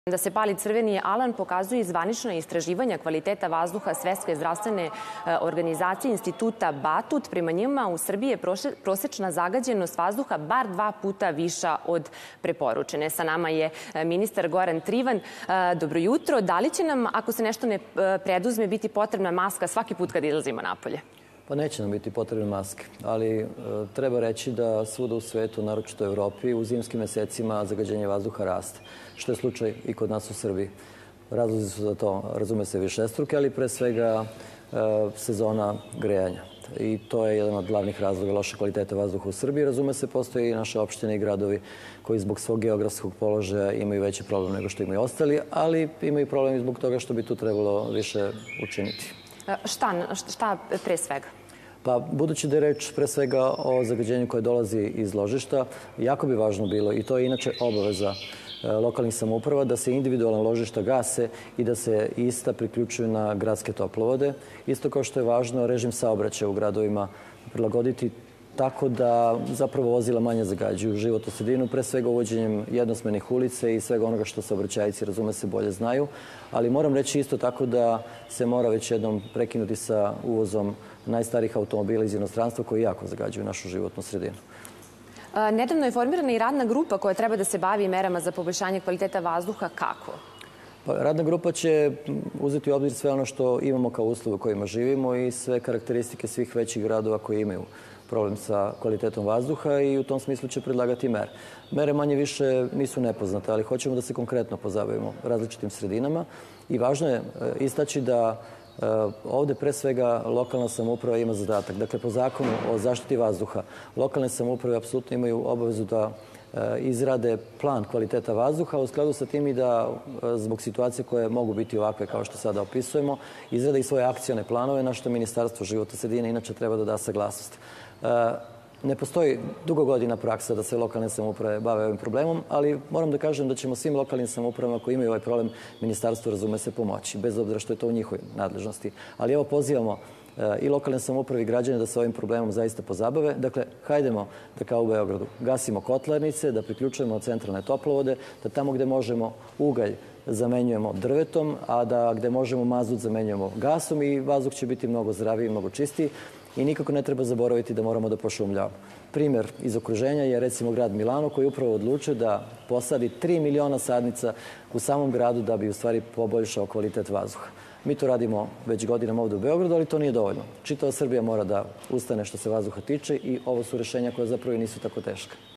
Akcioni planovi za čistiji vazduh, lokalne samouprave na potezu
Prethodnog vikenda zagađenost vazduha u Srbiji u nekim gradovima bila je i tri puta veća od dozvoljene. Nivo opasnih PM-10 čestica koji bi trebalo bi da bude od 50 do 75 mikrograma po metru kubnom, na Novom Beogradu bio čak 151. Ministar za zaštitu životne sredine Goran Trivan rekao je za RTS